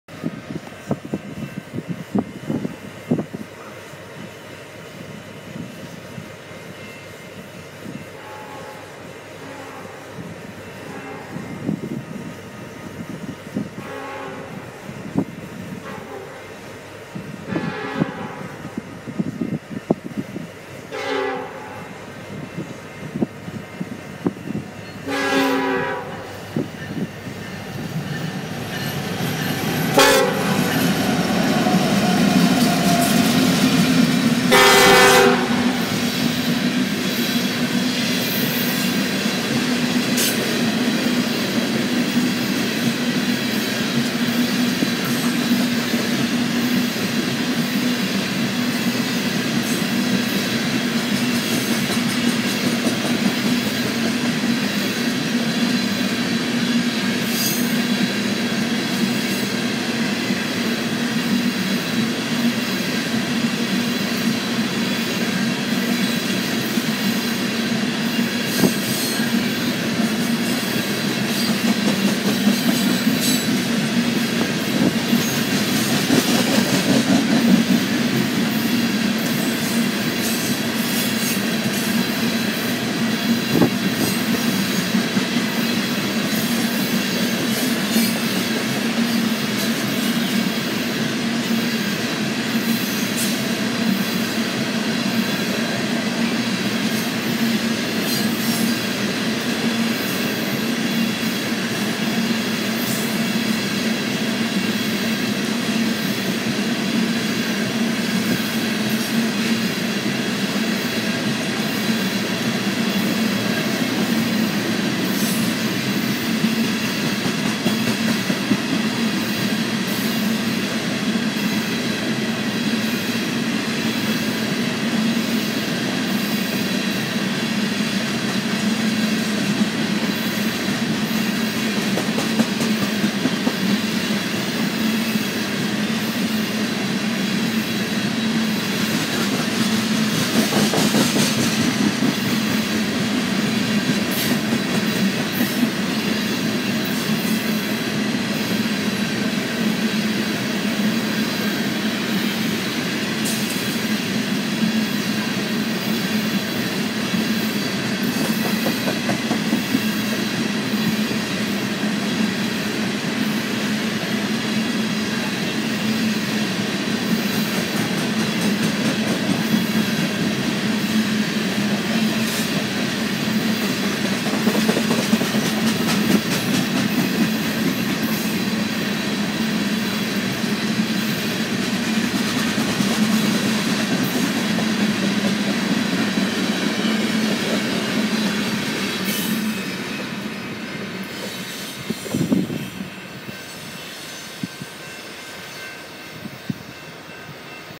BNSF 9215 (SD70ACe) leads a Eastbound BNSF coal train through Mendota, IL 01/30/22
After both Eastbound Amtraks came through Mendota, IL on the BNSF Mendota sub, a Eastbound BNSF coal train came through led by a pair of SD70ACes.
Eastbound BNSF coal train
BNSF 9215 (SD70ACe built 2008)
BNSF 8598 (SD70ACe built 7/2014)